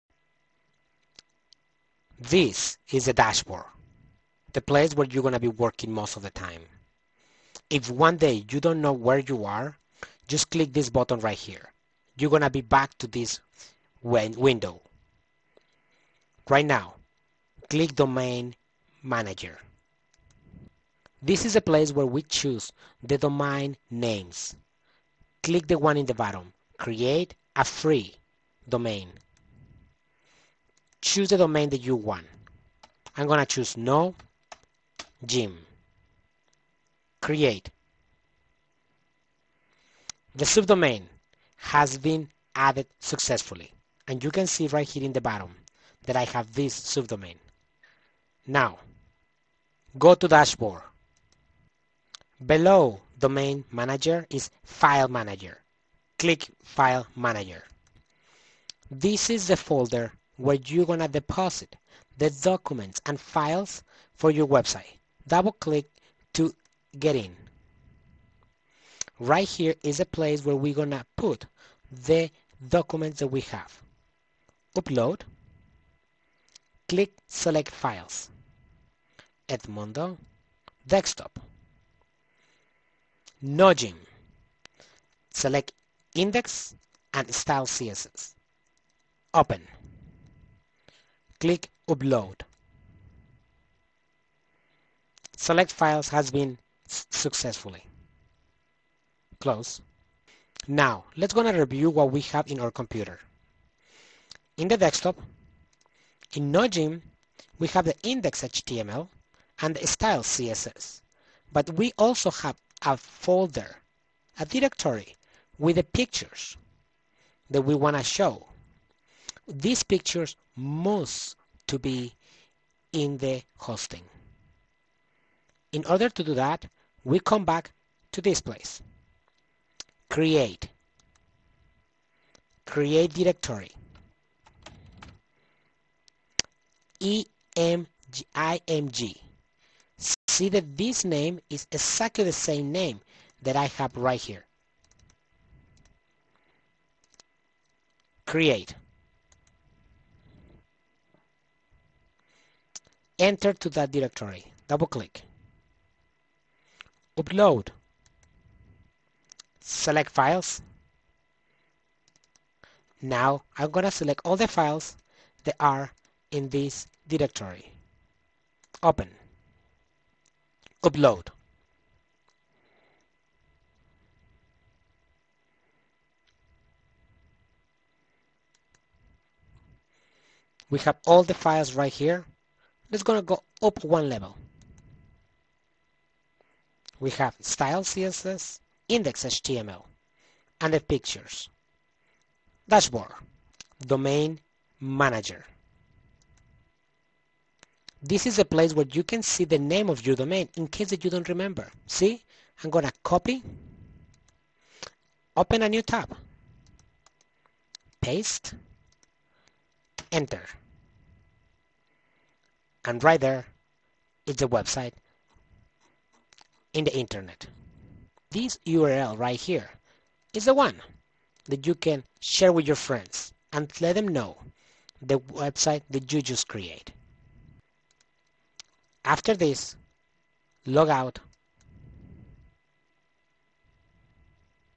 How to Upload files to - P2
Instructions in how to upload files to the free hosting account